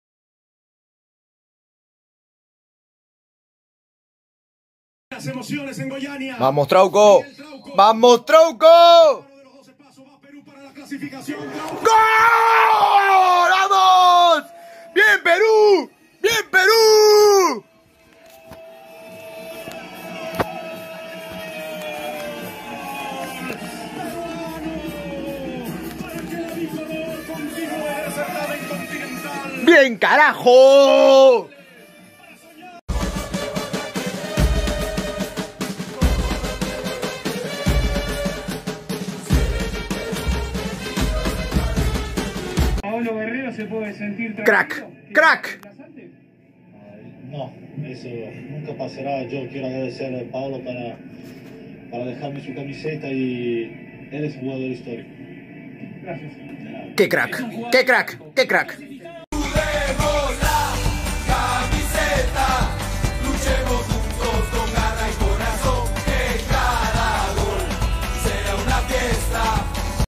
PERÚ vs. Paraguay 3-3 | Penal de Trauco y a la Semifinal | Copa America 2021 | 02/07/2021
Último penal, gol de Miguel Trauco y clasificación a la Semifinal de la Copa América